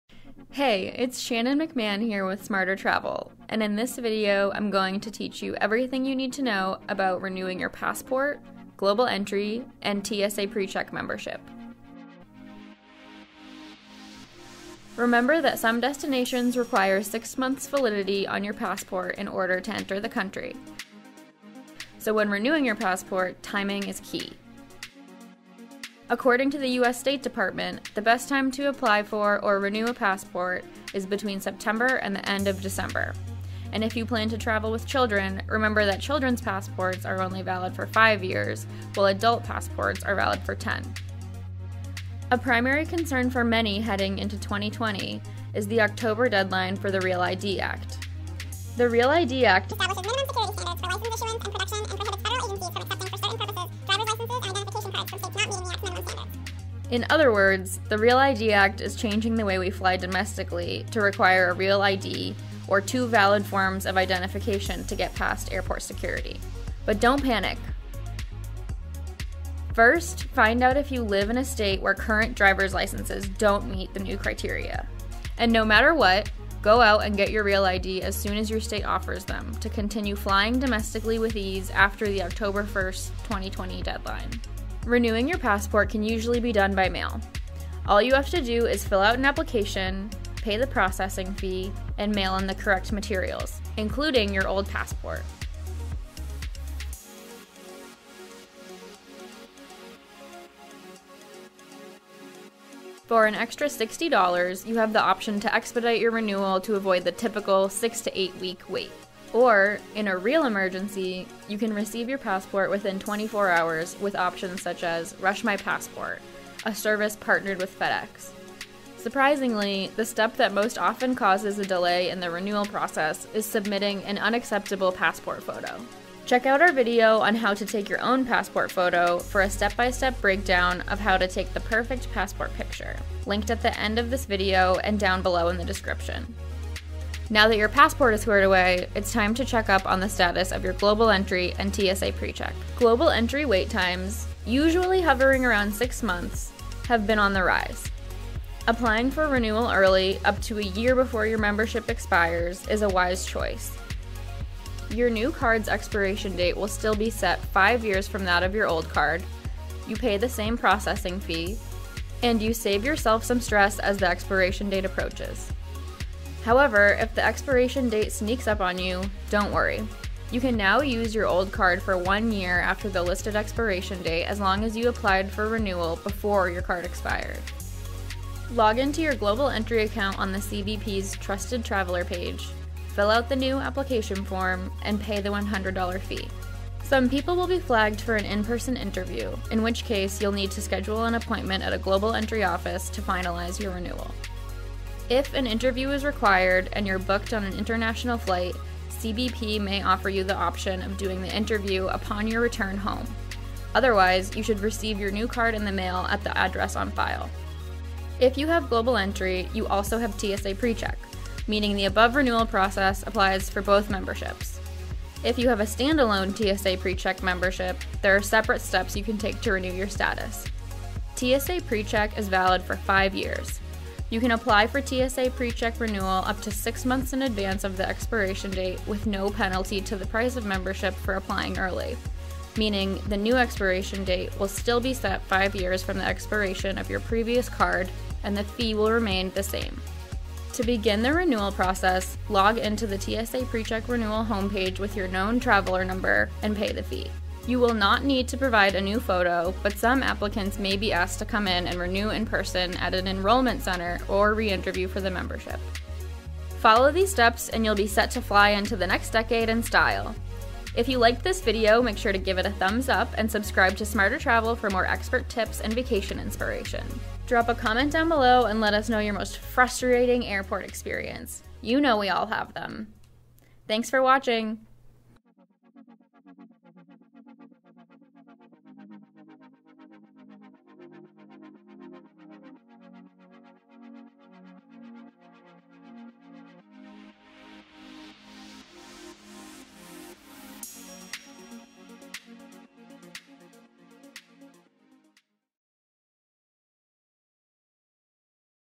How to Renew Your U.S. Passport, Global Entry, and TSA PreCheck | SmarterTravel
Overseas travel on your horizon? Not sure how to go about renewing your passport? Don’t worry - SmarterTravel has the expert tips and advice you need to make renewing your U.S. Passport, Global Entry, and TSA PreCheck membership a breeze.

From teaching you how to expedite your passport to taking your own passport photo, we break down U.S. passport renewal into manageable steps that will take the stress out of your travel preparations. While you're at it, remember to renew your Global Entry and TSA PreCheck membership to sail through airport security.

0:13 How to Renew Your U.S. Passport
2:20 How to Renew Your Global Entry
3:28 How to Renew Your TSA PreCheck Membership

Find more useful links and step-by-step instructions for renewing your passport, Global Entry, and TSA PreCheck